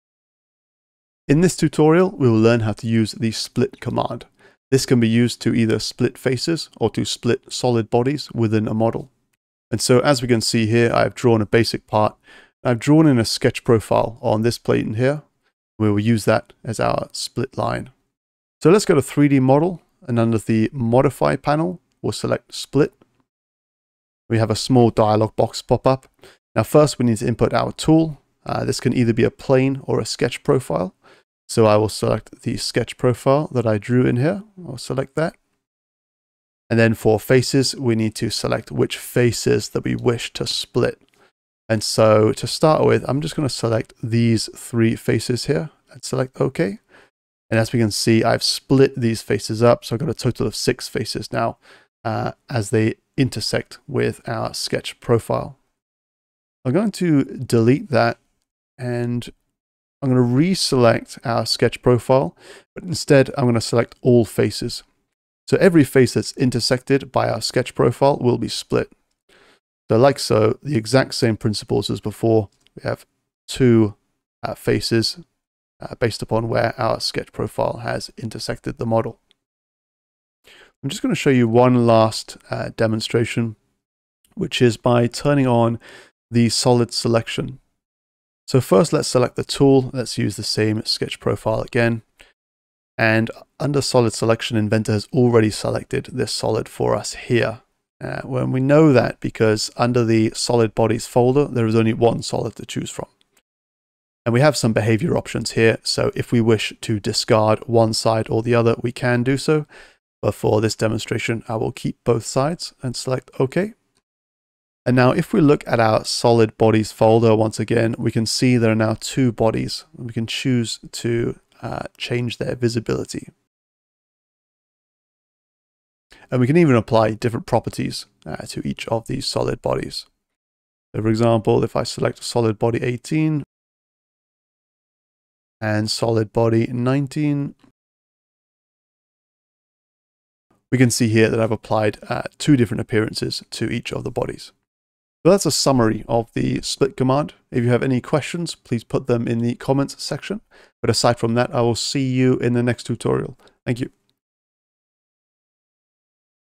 How to Split a Part | Autodesk Inventor 2022 | Basic Tutorial
Learn how to Split a Part using the Split command. This command can split either faces or entire solid bodies to ease workflow and enable more geometries for specific design features.

🕘Timestamps:
00:00 Introduction
00:18 Split Command
00:38 Split Faces
01:20 Split Solid Bodies

If you have any questions or requests, please post in the comments, I will respond as soon as I can.

Thank you, let's go and Design! ✅